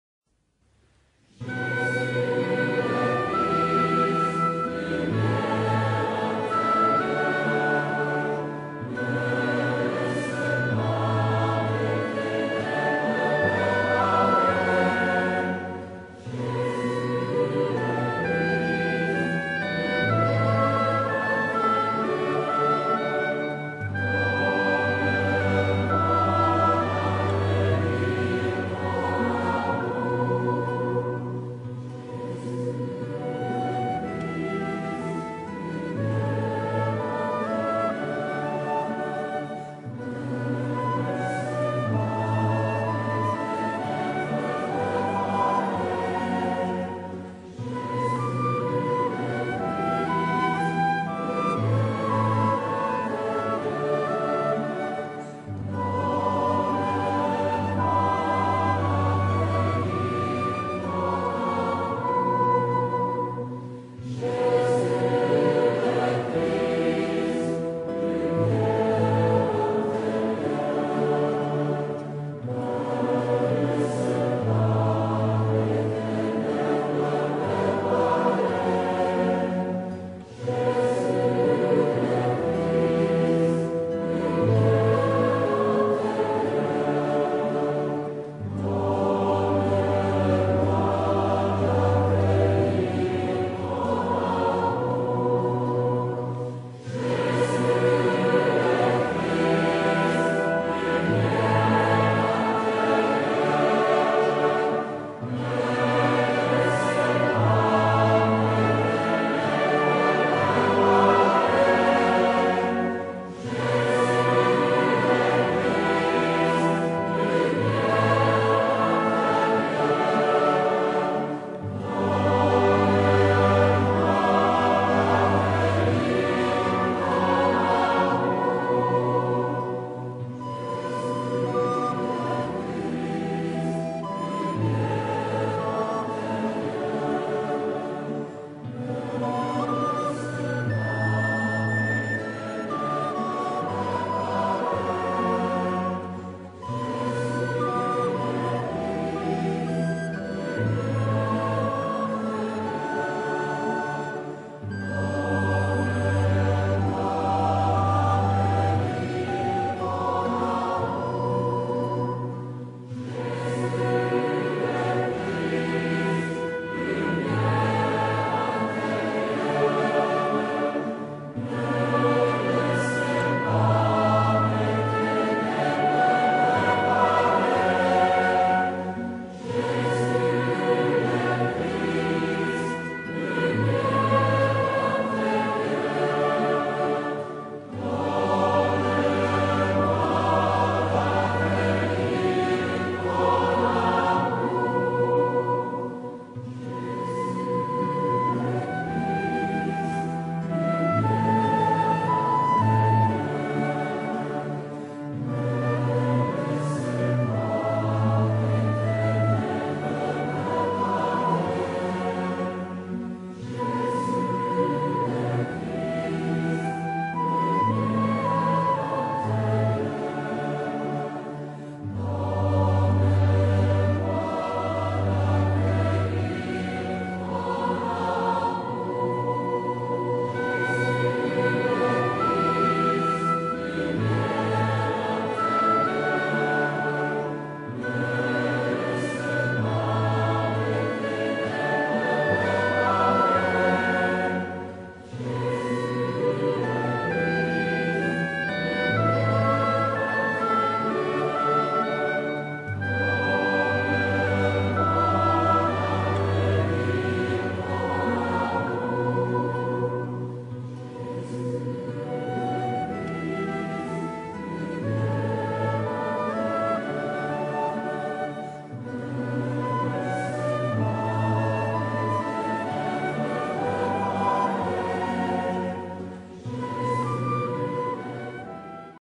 prière pour la conversion des musulmans.mp4
Sur les gros grains, vous direz (dire 1 fois) :
Par les Saintes Plaies de Jésus et les Larmes de Sa Sainte Mère.
Sur les petits grains, vous direz : (dire 10 fois) :
Doux Père, faites-leur connaître Votre Fils.

Oui, Mon père est DOUX, et cette appellation qu'enfin les hommes Lui attribueront et particulièrement les Français, bouleversera Ses Entrailles !
Oui, Mon père est DOUX, et cette affirmation guérira les cœurs blessés de ceux qui la prononceront !